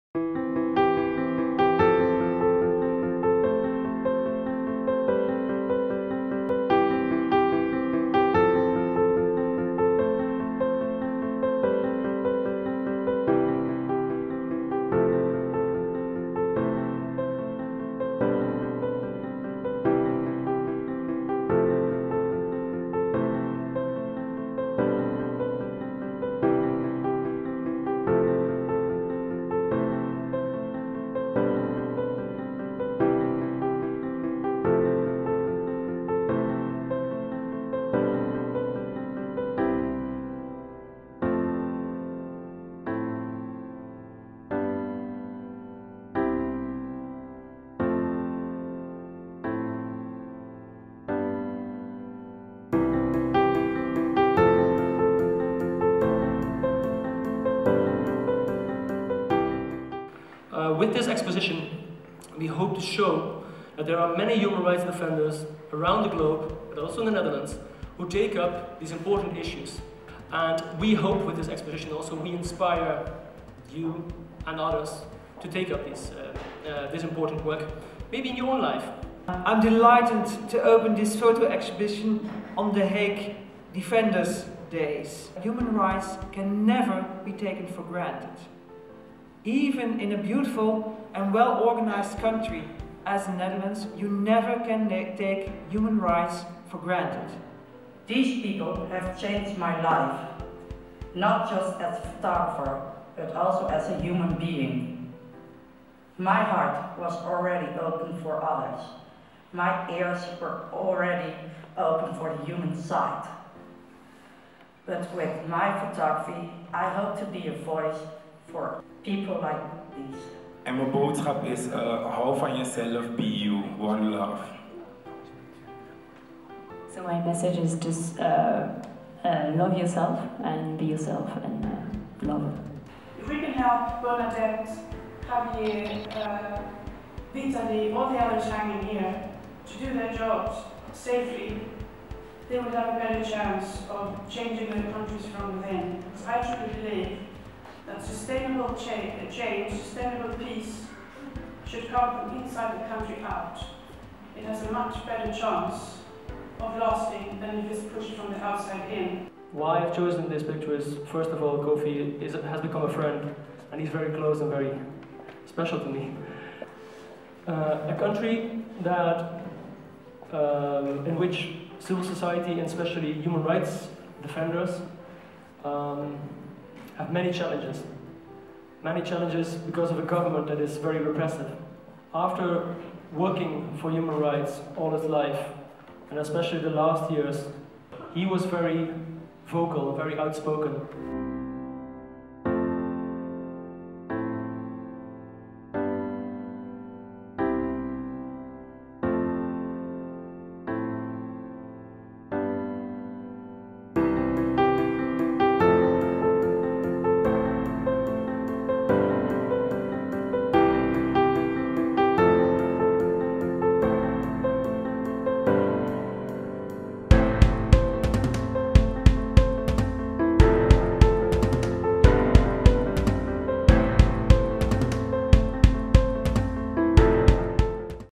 Photo exhibition We are all human rights defenders Justice and Peace Netherlands
In December 2016 Justice and Peace developed and hosted this photo exhibition with portraits of international and Dutch human rights defenders made by photographers Anette Brolenius and Daniella van Bergen. We are all human. We all have rights. We can all be a human rights defender.